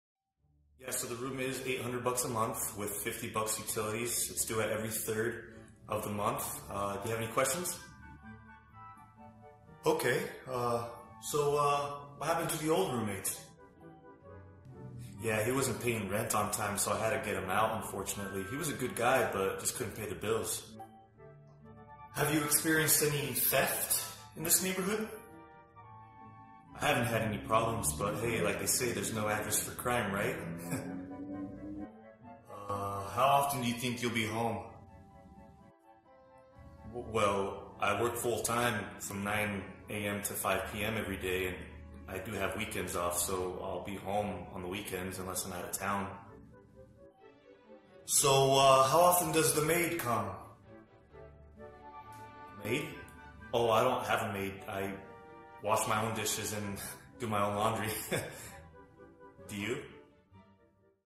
Roommate Search Nightmare
Fabian needs to replace his old roommate Jaime but everyone seems too uptight...